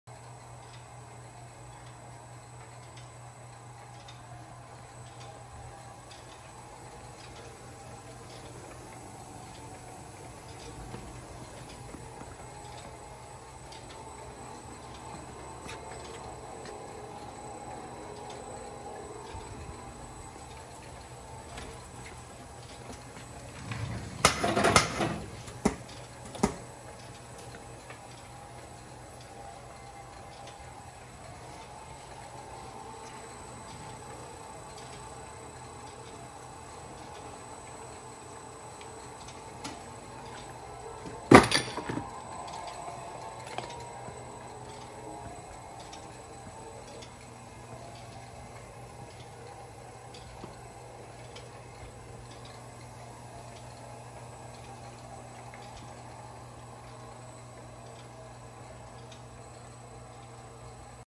Pearson Hot Glue Case Sealer
Used, Pearson Hot Glue Top Cawe Sealer SN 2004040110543, offered by Litchfield Packaging Machinery Corporation, Morris Connecticut.